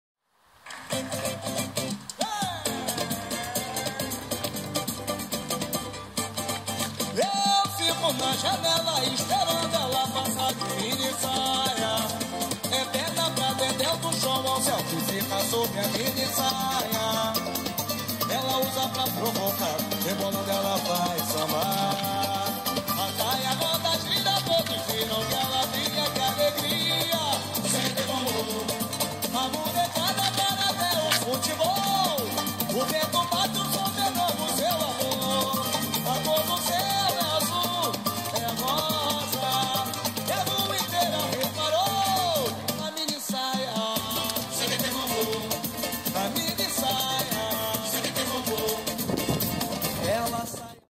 Brazilian Samba - Mishel Jemma Finlayson
Rio Carnival Muse Mishel performs in Europe. Music by Dudo Nobre.